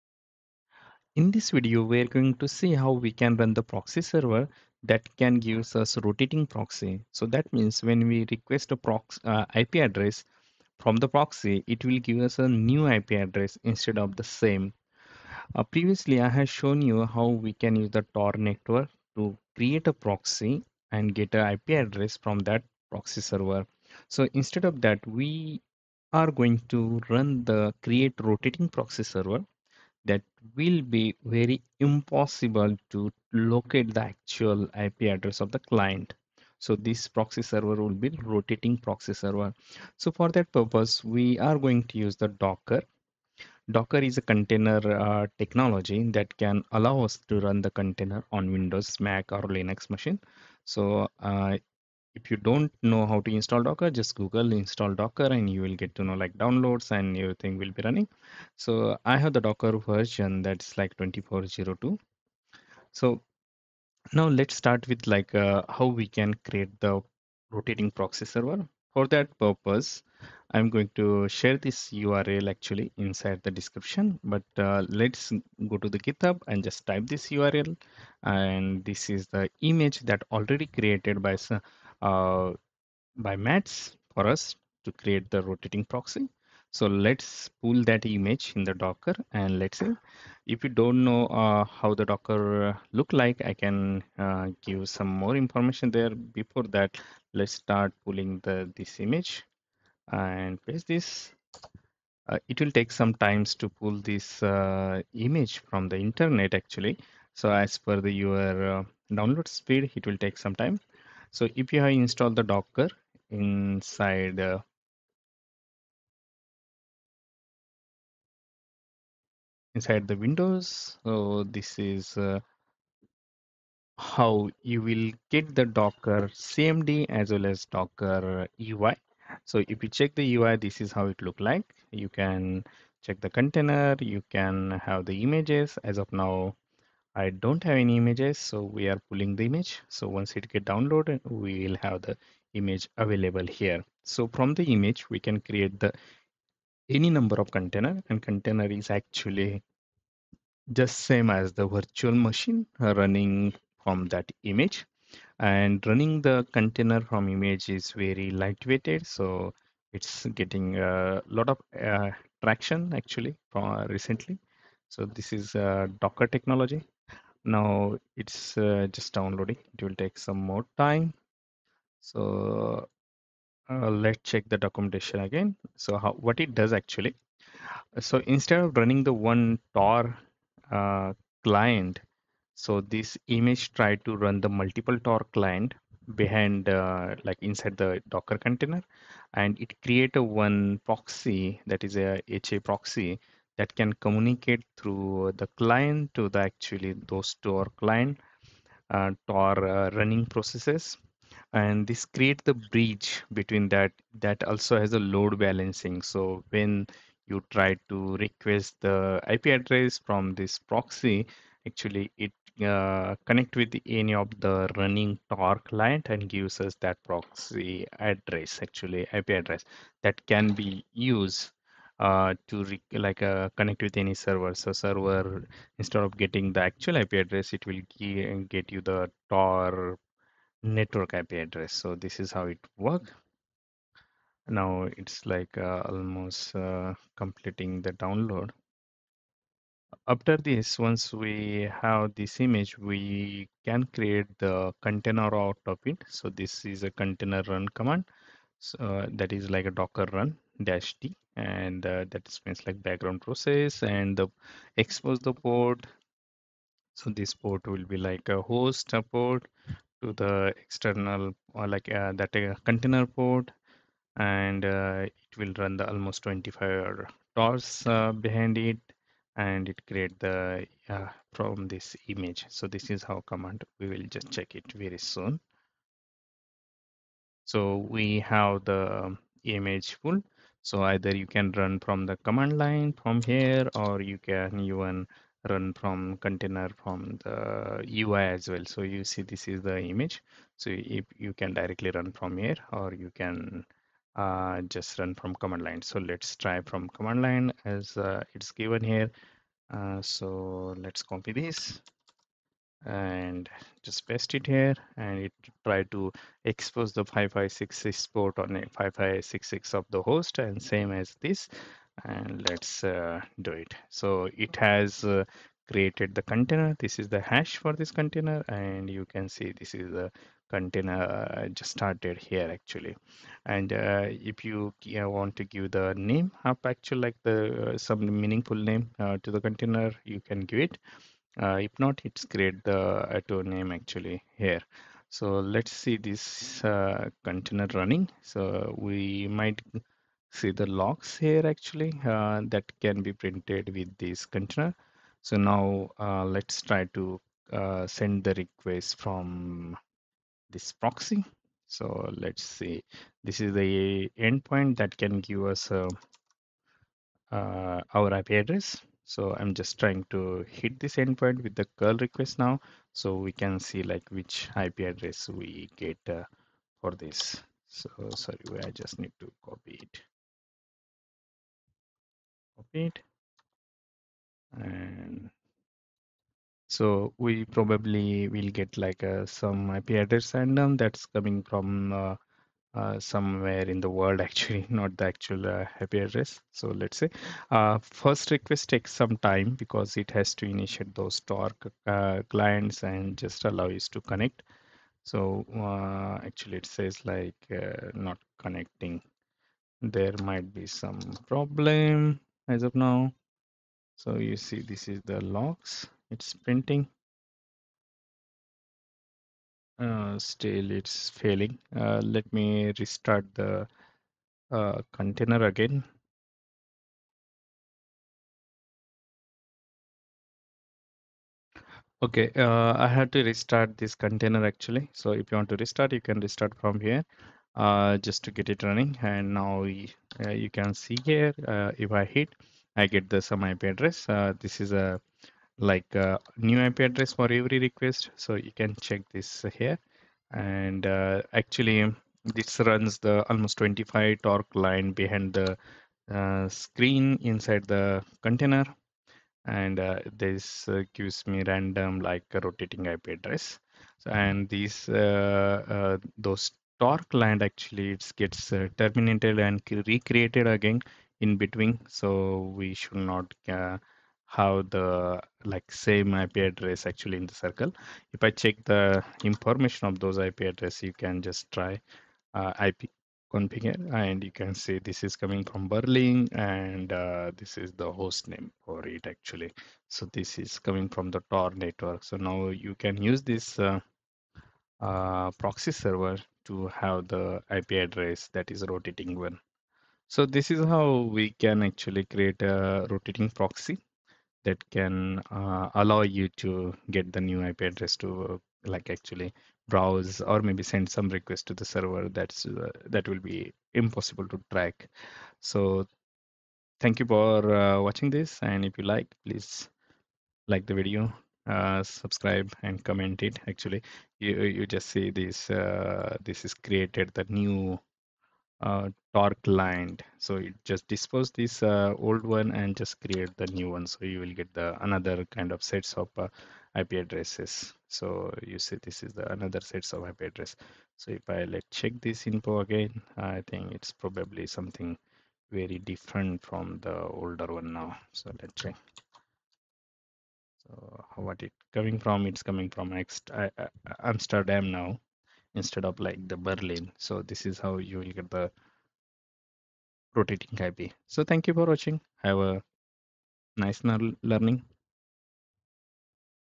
Create rotating proxy server from tor browser client
If you are looking for a rotating proxy which will give new IP for every request, this will be the lifesaver to create a rotating proxy server with TOR network inside a docker container.